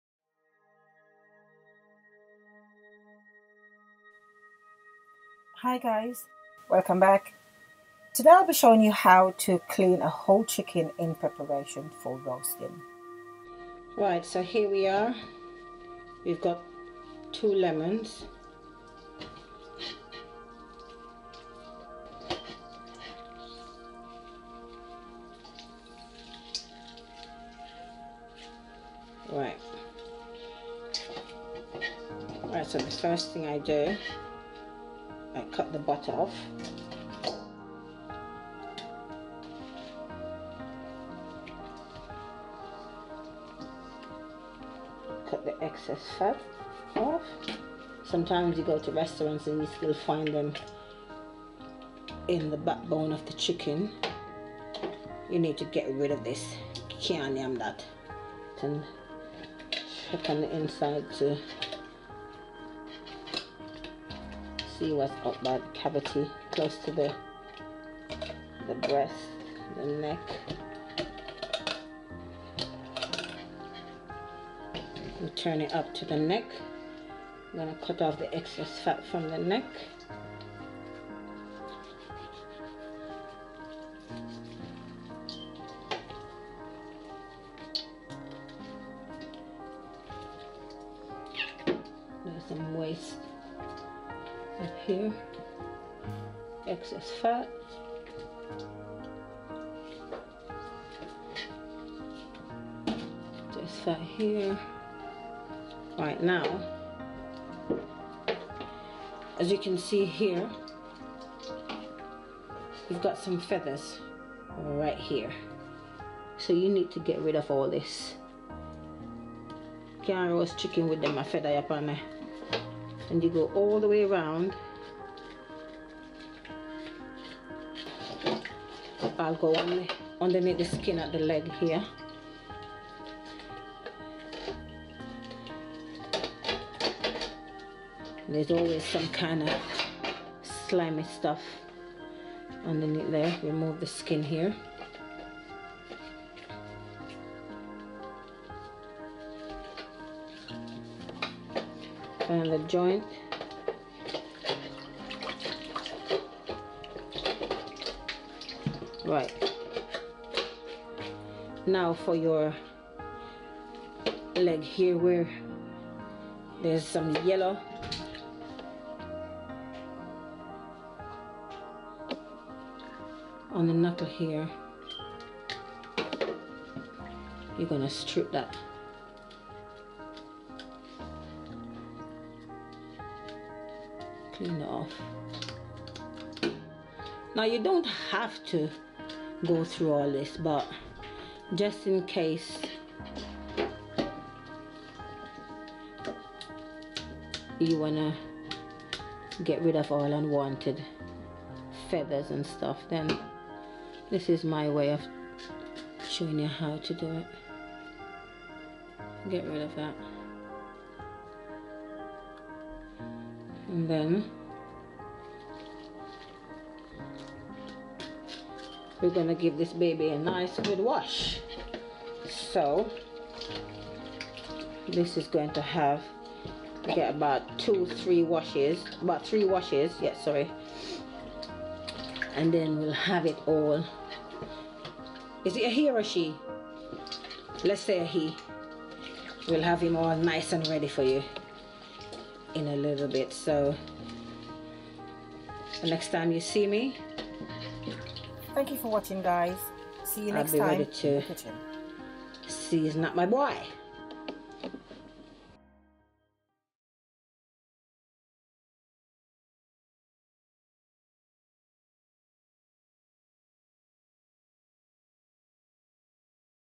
How to clean a whole chicken for roasting
Hi. Thank you for dropping by. If this is your first time here, I hope you enjoy the video.

Ingredients:
1 Whole chicken
Lemons or vinegar for washing